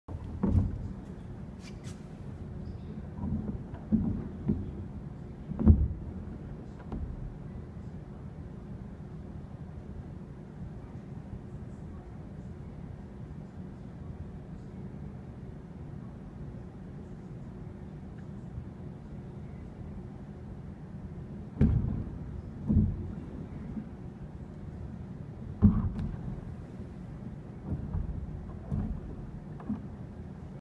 Polar Bear Enrichment
Polar Bears in the wild will rise on their back legs and pounce on the roof of a seal den to get their food. This behavior can be seen at Como when large white barrels are placed in their exhibit for enrichment. ‪#‎ComoZoo‬